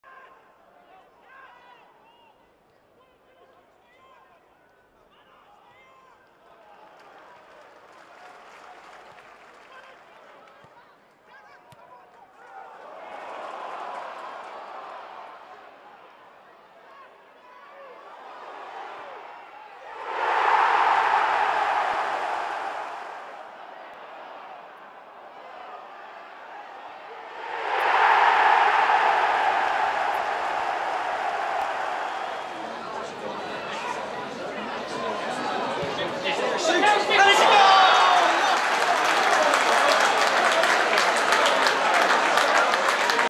Luton Town goal of the month: August 2015
Vote for your best.
A - Jack Marriott  v Bristol City
B - Cameron McGeehan v Oxford
C - Craig Mackail-Smith v Yeovil